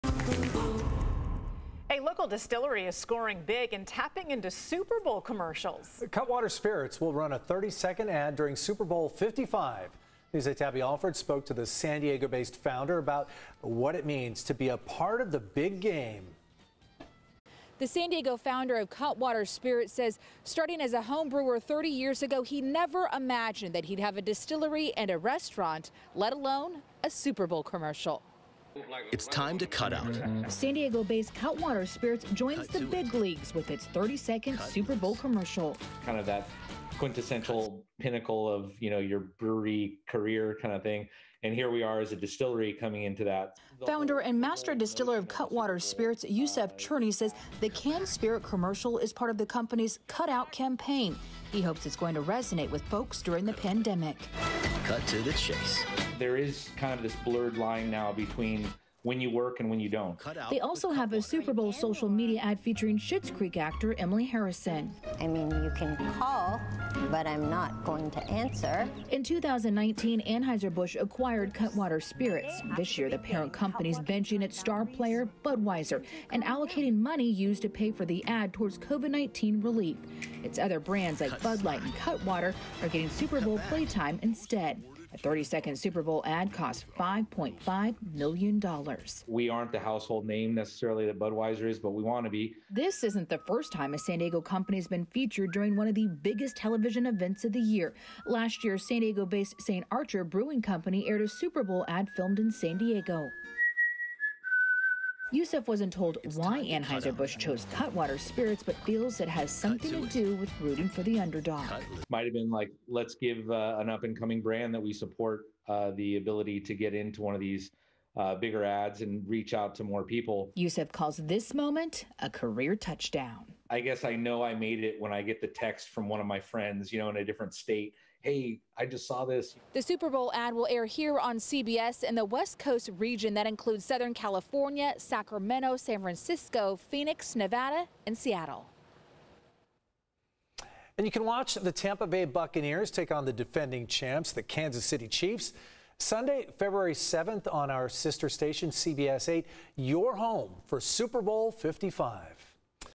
San Diego distillery scores big with Super Bowl commercial
A Super Bowl ad costs millions of dollars, which some companies say is worth it to put San Diego in the spotlight.